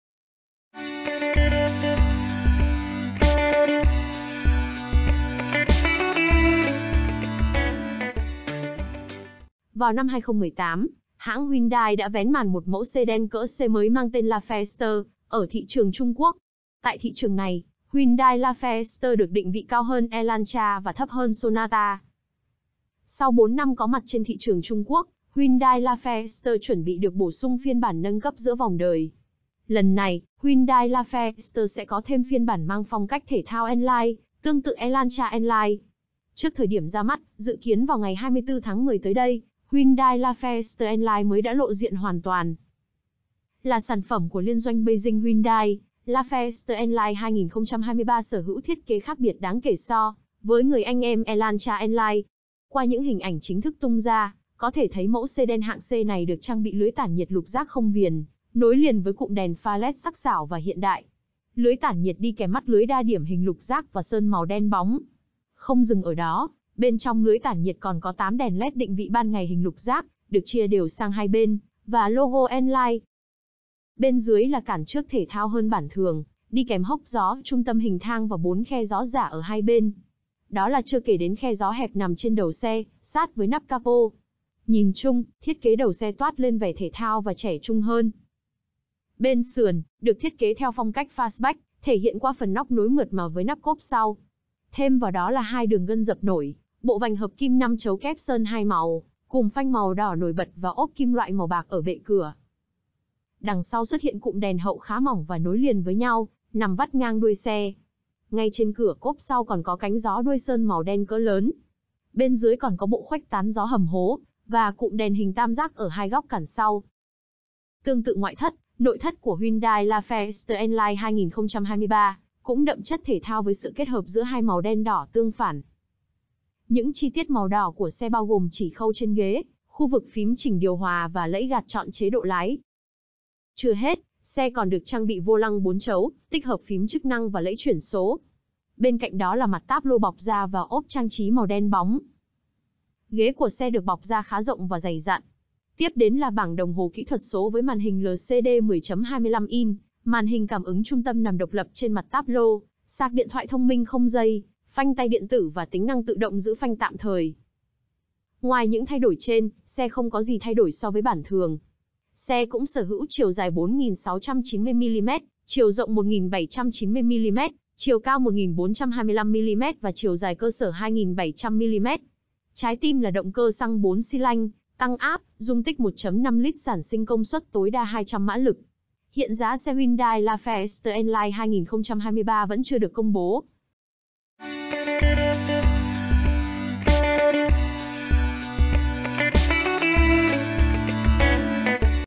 Hyundai lại nhá hàng mẫu xe mới có thiết kế "đẹp miễn chê" tiện nghi long lanh mong chờ "giá sẽ rẻ"
Là sản phẩm dành cho thị trường Trung Quốc, Hyundai Lafesta N Line 2023 sở hữu thiết kế khác biệt đáng kể so với người anh em Elantra N Line.